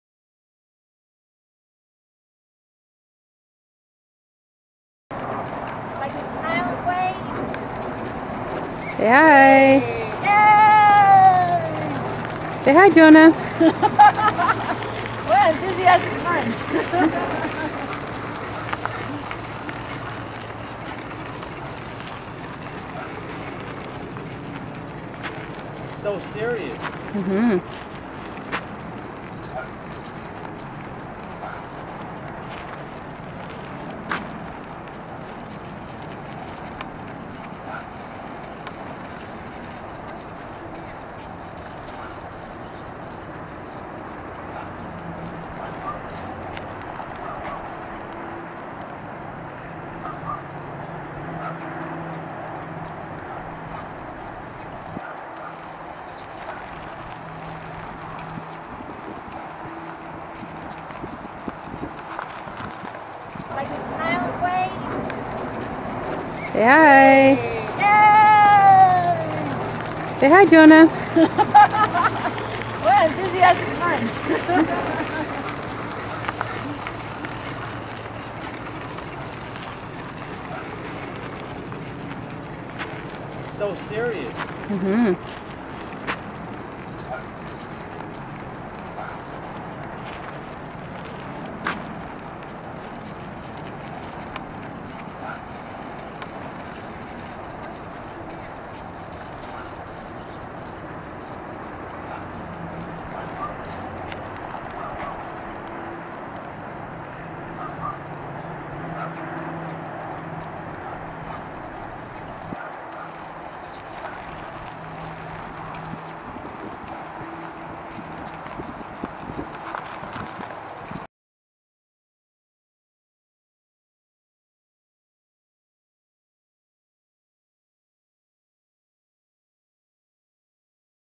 Cherries - available for parties- cart driving with kids
To book your party or special event or movie go
Cherries the Miniature Trick Horse drives her cart with Nancy at the reins, and two kids as passengers.
Cherries is available for parties, special events, weddings, promotions, grand openings, senior homes, and of course, special charities .